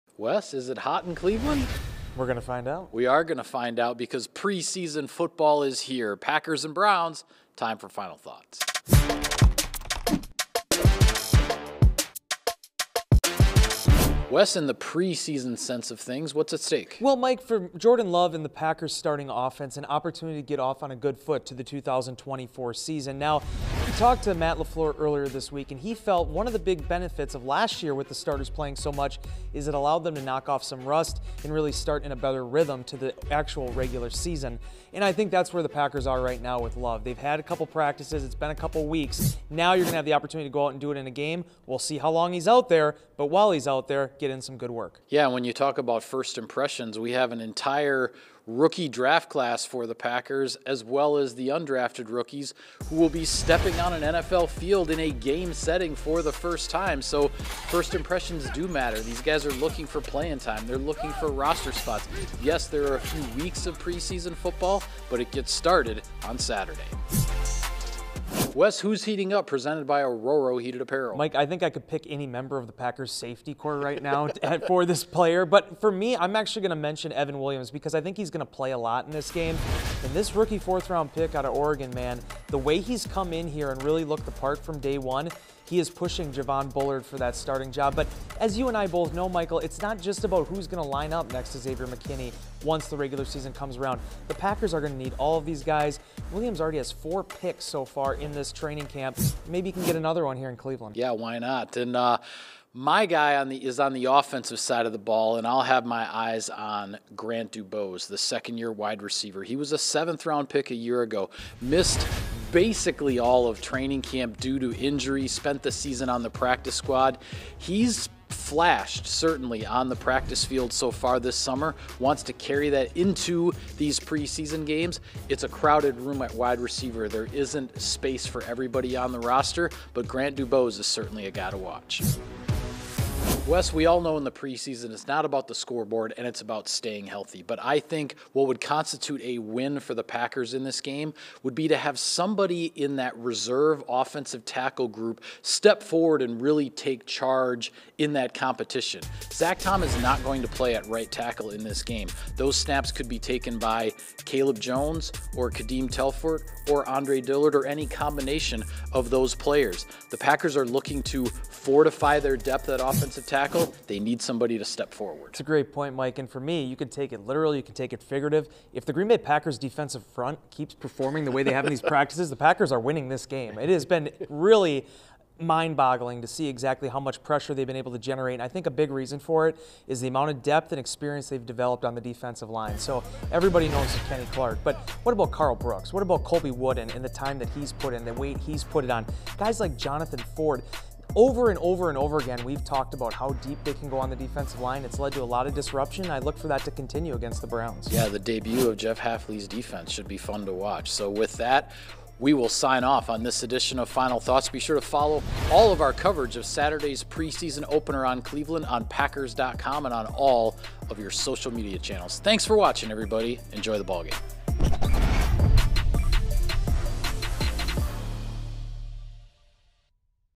Final Thoughts: Packers at Browns
Wes Hodkiewicz and Mike Spofford break down the keys to the game as the Green Bay Packers take on the Cleveland Browns in their preseason opener.